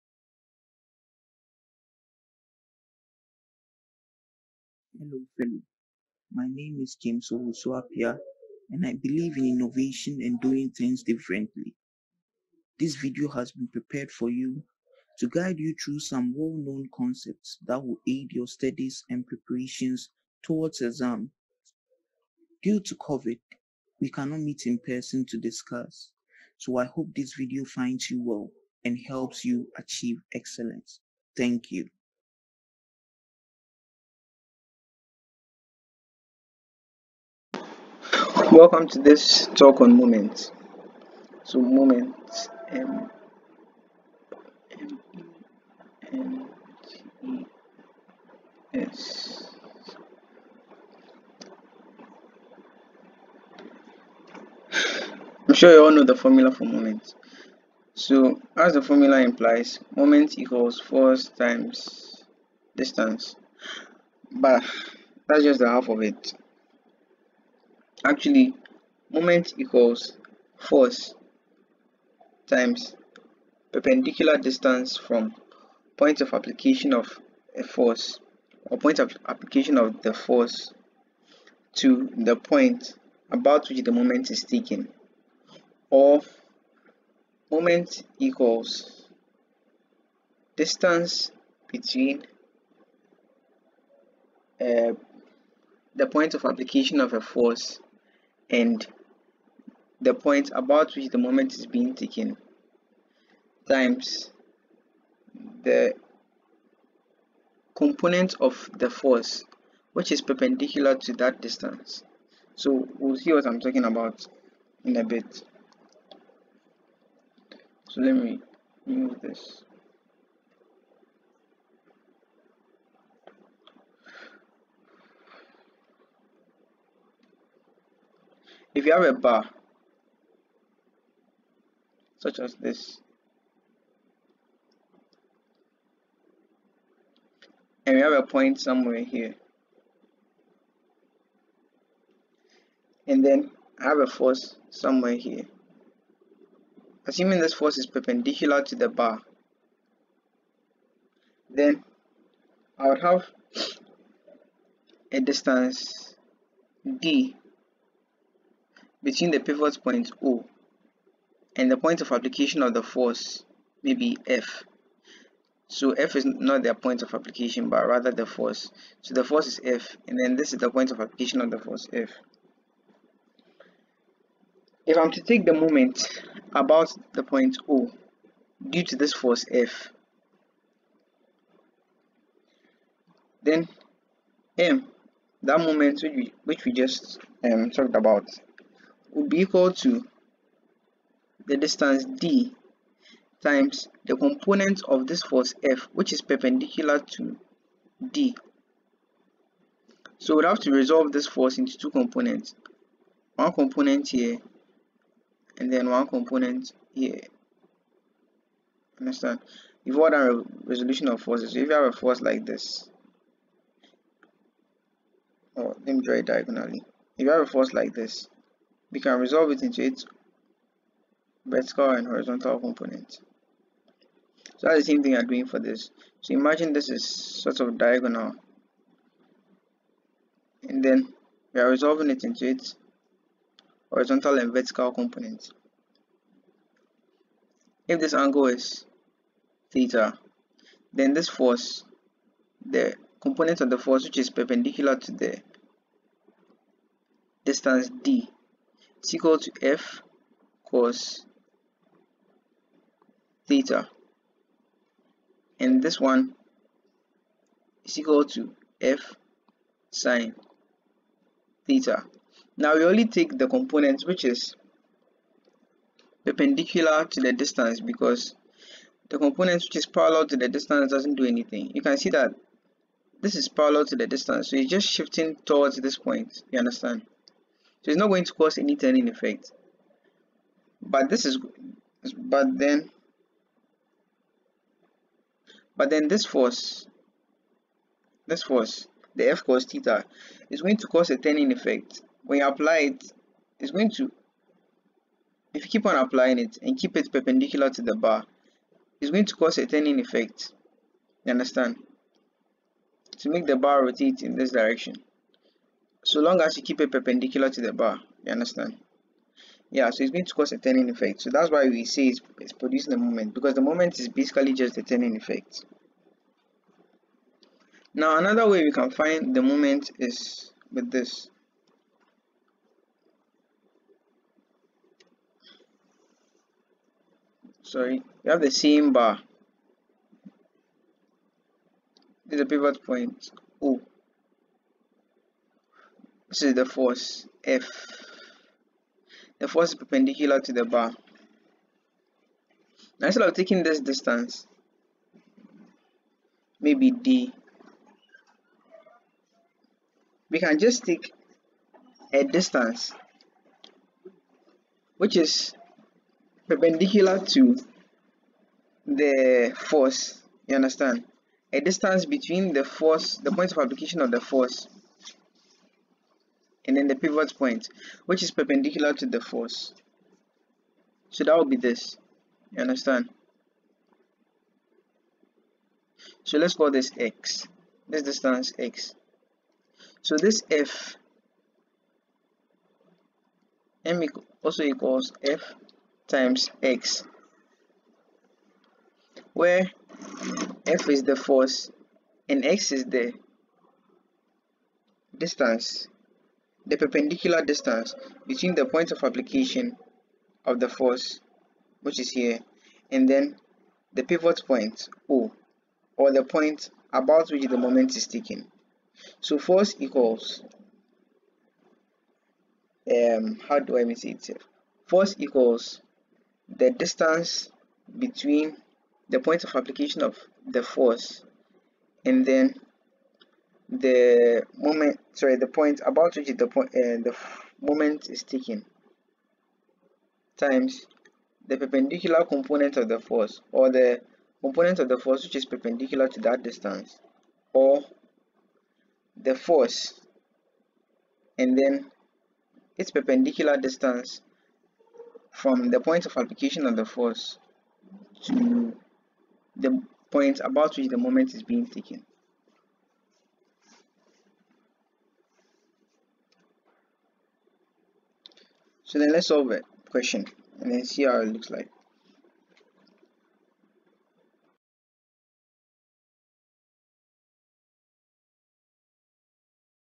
Moments
A quick lecture on the moments of vectors from the definition, what it is used for, and how important it is.  The questions come in the next videos to be uploaded soon. This video is prepared with your success in mind. A James Owusu-Appiah Production.